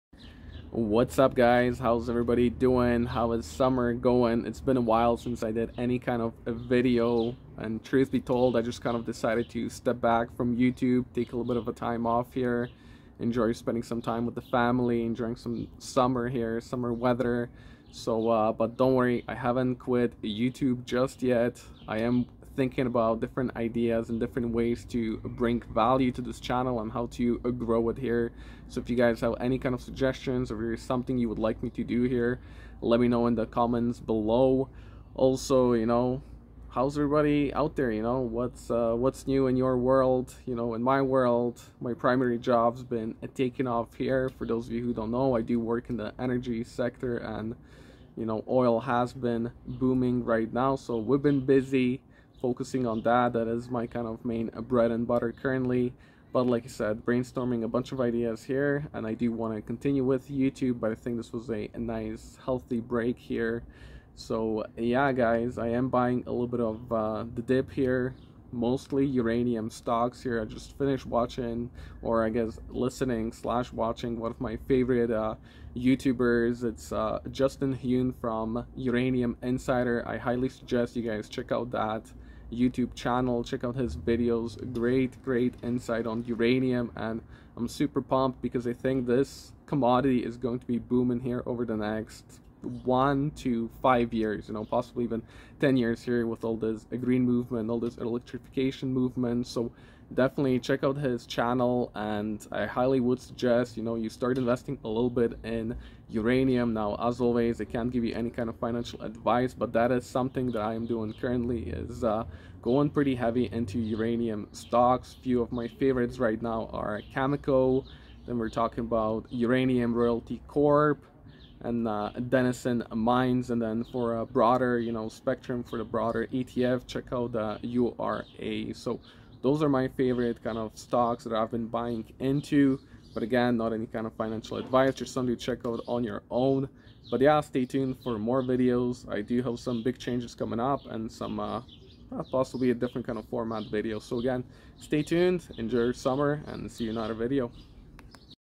How's ur SUMMER? ☀️🏞
How is everyone doing? How is ur summer? I took a small brake from YouTube but getting back at it here. I've spend these last 4 weeks brainstorming some ideas on how to bring more value to this channel. Looking forward to executing these ideas; so look for some different format coming up soon. Otherwise I hope ur doing well and if you have any suggestions on how to improve this channel or things you would like me to discuss leave me a comment bellow. Cheers guys! 🤙🏼